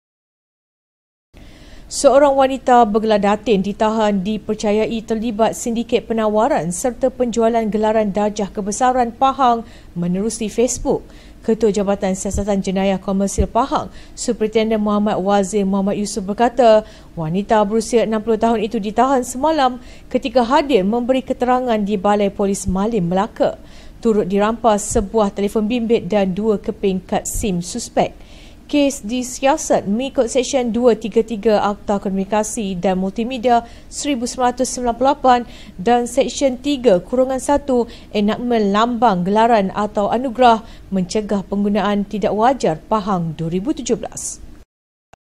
Datin Terlibat Jual Darjah Kebesaran Palsu Negeri Pahang
Seorang wanita bergelar Datin ditahan dipercayai terlibat sindiket penawaran serta penjualan gelaran darjah kebesaran Pahang menerusi Facebook.

Ketua Jabatan Siasatan Jenayah Komersial Pahang, Superitendan Mohd Wazir Mohd Yusof berkata wanita berusia 60 tahun itu ditahan semalam ketika hadir memberi keterangan di Balai Polis Malim, Melaka.

Turut dirampas sebuah telefon bimbit dan dua keping kad sim suspek.

Kes disiasat mengikut Seksyen 233 Akta Komunikasi dan Multimedia 1998, dan Seksyen 3(1) Enakmen Lambang, Gelaran atau Anugerah (Mencegah Penggunaan Tidak Wajar) Pahang 2017.